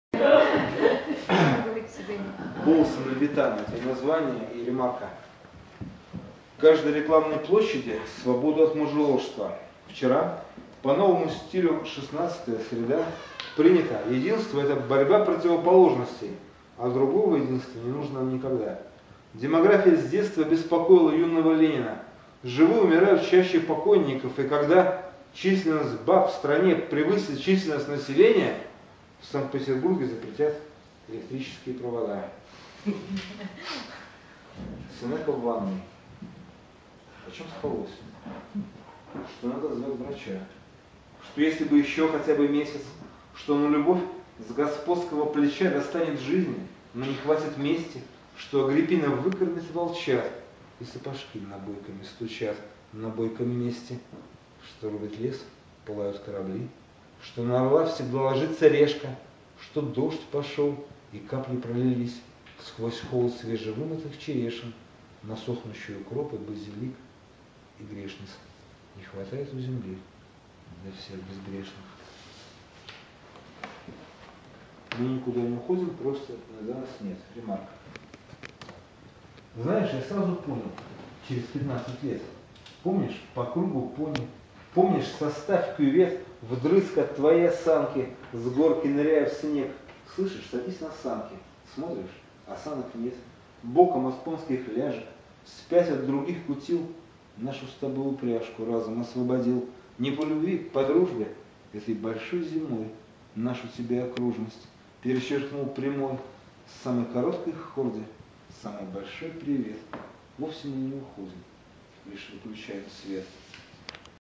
12-12-2011: Зверевский центр - Михаил Свищёв
Москва, 2011

Зверевский центр современного искусства. выступали мои старые знакомые, товарищи, даже друзья, связанные с сайтом "Термитник поэзии"...

на этом видео - Миша Свищёв читает свои стихи. у меня особенное отношение к поэзии Свищёва... открылась она мне много лет назад... Михаил пишет относительно немного, но мне его творения весьма близки - по некоторой глубине что ли... иногда сложно подобрать слова... да и надо ль их подбирать???